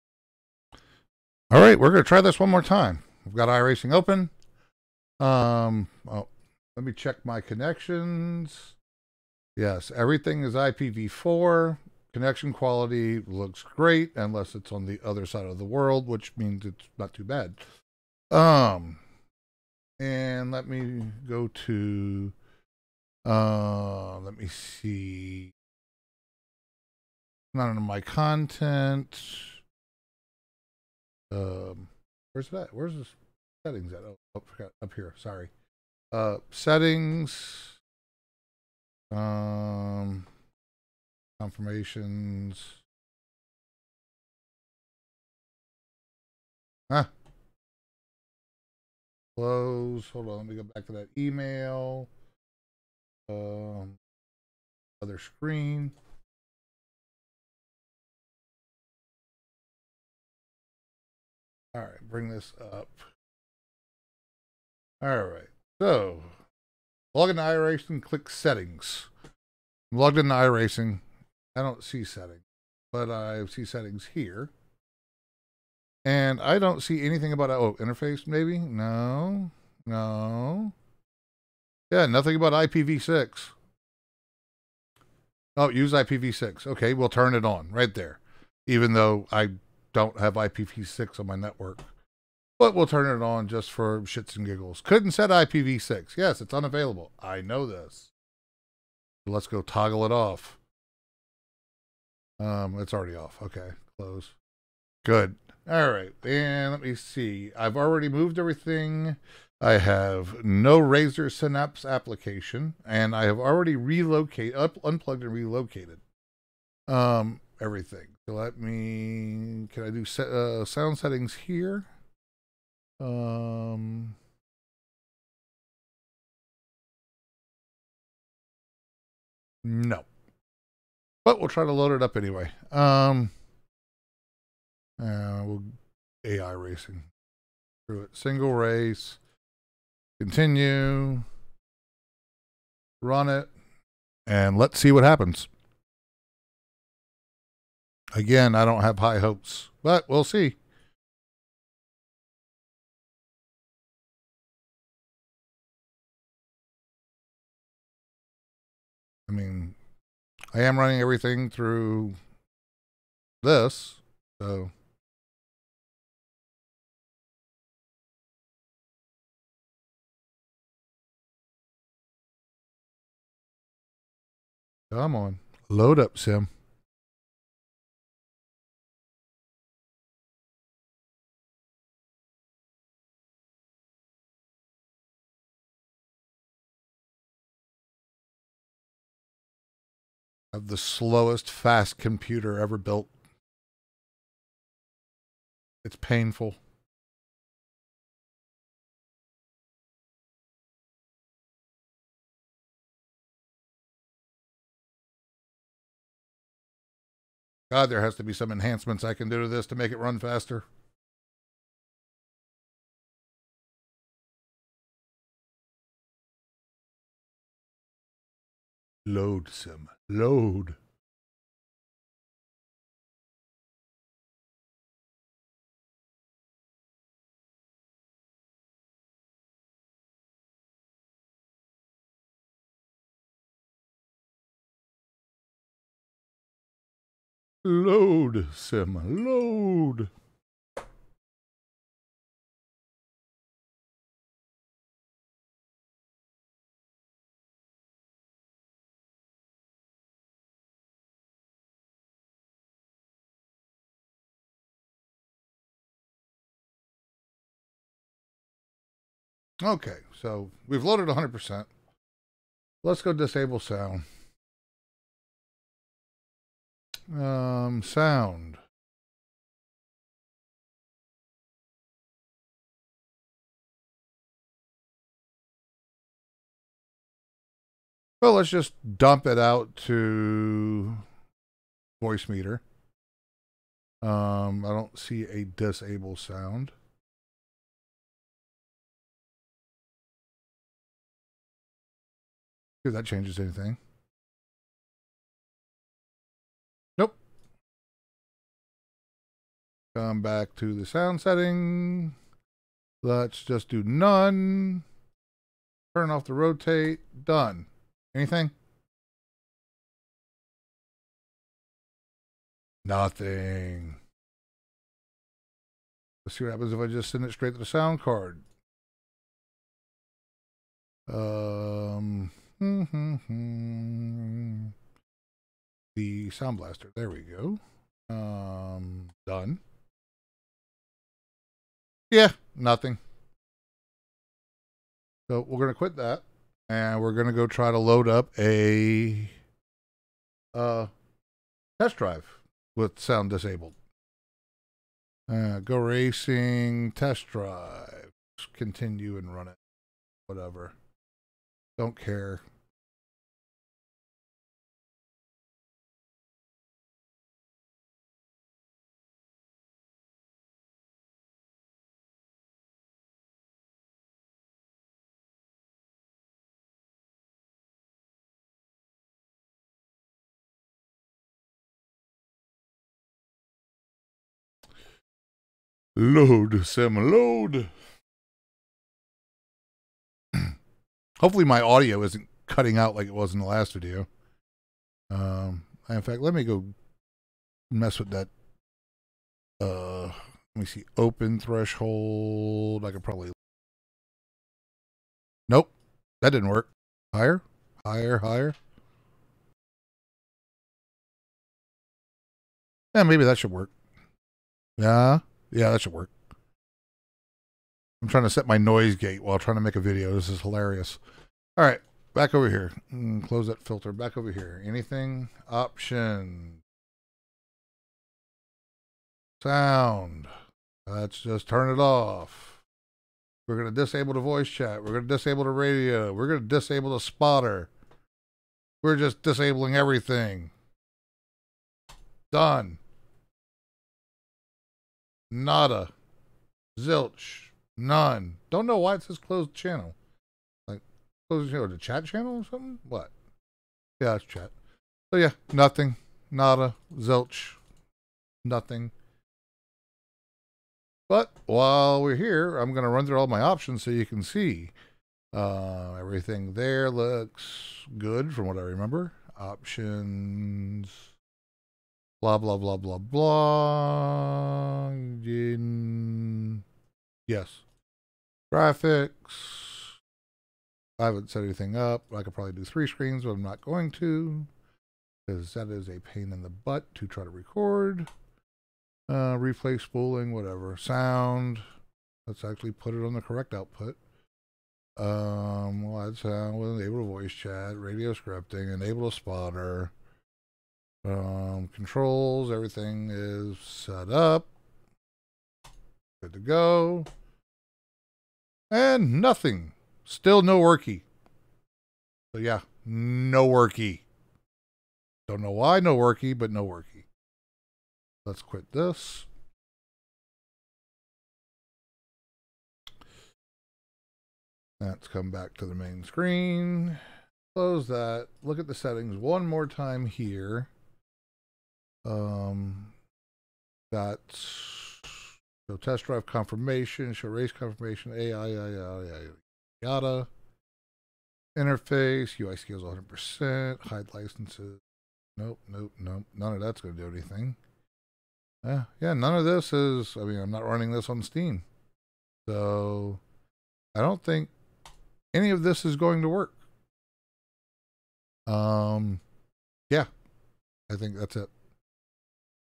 iRacing still no workie...
Tried all the things. Rebooted the computer. iRacing still no workie. This makes Greg a very sad boy. :(